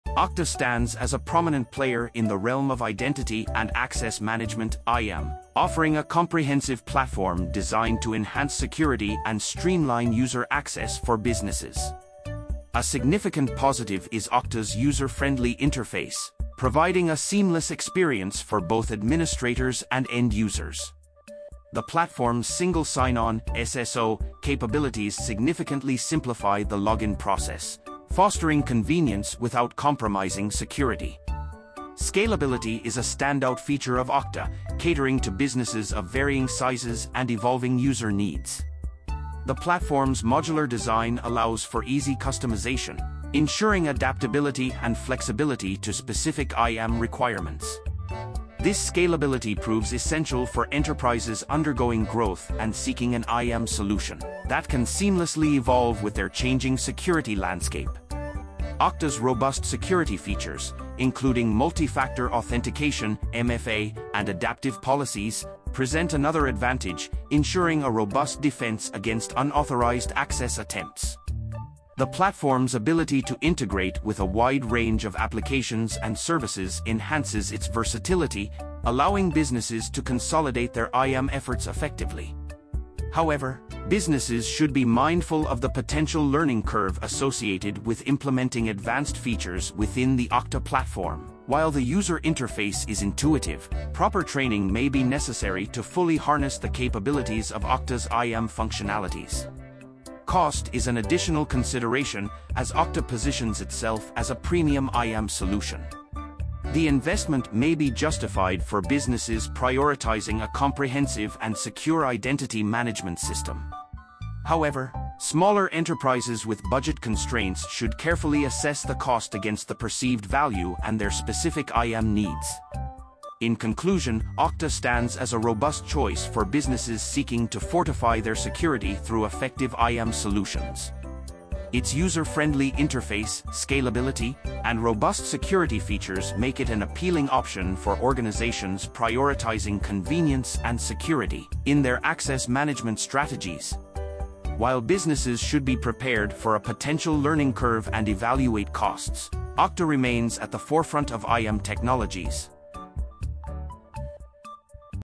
🔥 Okta Review: Pros and Cons
Okta emerges as a leader in the Identity and Access Management (IAM) landscape, providing businesses with a robust platform to enhance security and streamline access management. A significant positive is Okta's commitment to security, offering a multi-layered defense system that includes Single Sign-On (SSO), Multi-Factor Authentication (MFA), and adaptive policies. This ensures a comprehensive and adaptive approach to safeguarding user identities and data.

Scalability is a notable strength of Okta, addressing the diverse needs of businesses with varying user bases. The platform's modular architecture enables seamless customization, ensuring adaptability and flexibility to specific IAM requirements. This scalability proves essential for enterprises experiencing growth and seeking an IAM solution that can effortlessly scale with their evolving security needs.

Okta's seamless integration capabilities with a wide array of applications and services present another advantage, providing businesses with a unified IAM solution. This versatility streamlines access management efforts, allowing organizations to consolidate their security measures effectively.

However, businesses should be aware of the potential complexity associated with configuring and optimizing advanced IAM features within the Okta platform. While the platform prioritizes security and usability, users may need to invest time in training to harness the full potential of Okta's IAM functionalities effectively.

Cost is an additional consideration, as Okta positions itself as a premium IAM solution. The investment may be justified for businesses prioritizing a comprehensive and secure access management system. Nonetheless, smaller enterprises with budget constraints should carefully evaluate the cost against the perceived value and their specific IAM requirements.

In summary, Okta IAM Solutions stand as a reliable choice for businesses aiming to elevate their access management and security protocols. Its robust security features, scalability, and seamless integration capabilities make it an attractive option for organizations prioritizing a secure and streamlined IAM strategy. While businesses should be prepared for potential complexity and evaluate costs, Okta remains a leader in the realm of IAM technologies.